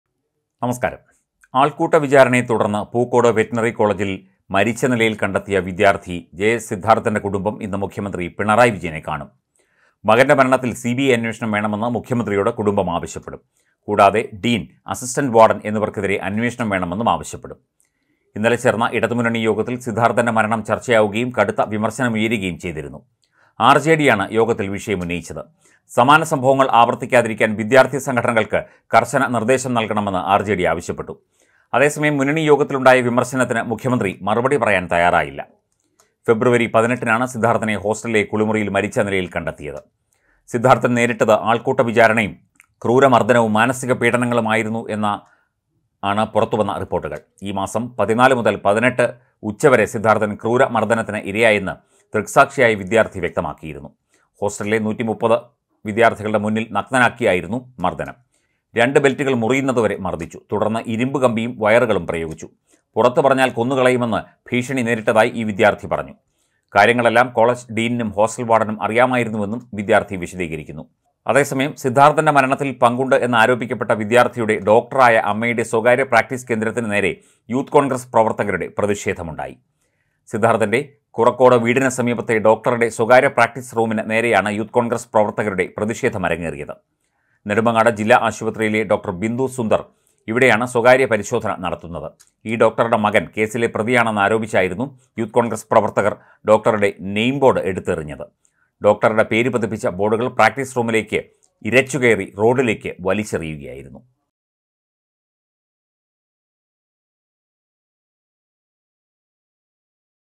സിബിഐ അന്വേഷണം ആവശ്യപ്പെട്ട് സിദ്ധാർത്ഥന്റെ കുടുംബം ഇന്ന് മുഖ്യമന്ത്രിയെ കാണും | Siddharth
സിദ്ധാർത്ഥന്റെ മാതാപിതാക്കൾ അൽപ്പസമയത്തിനകം പിണറായി വിജയനെ കാണും